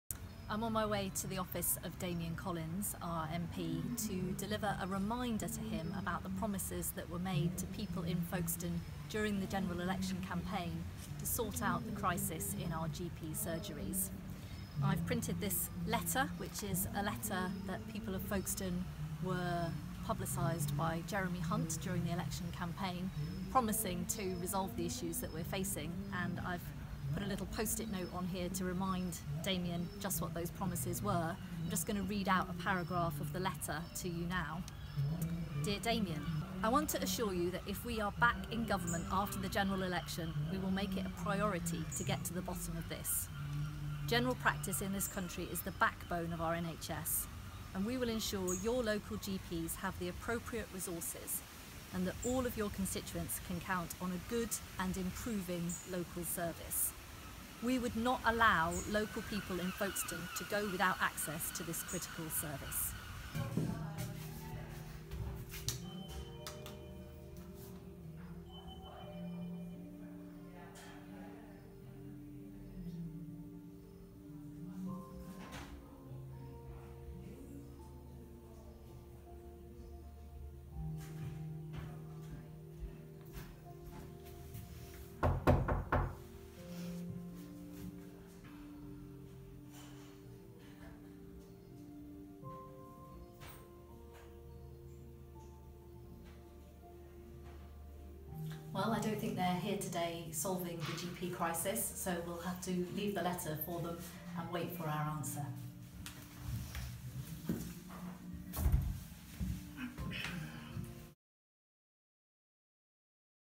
Folkestone GP crisis – a letter for Damian Collins MP
Folkestone is facing an unprecedented crisis in GP provision as a busy surgery closes. Other surgeries in the town have closed their lists due to fears for patient safety in the overstretched service, leaving nearly 5000 people without care.

Labour GE2017 candidate Laura Davison delivers a reminder to Damian Collins MP of the Conservatives' promises to ensure nobody in the town goes without a GP.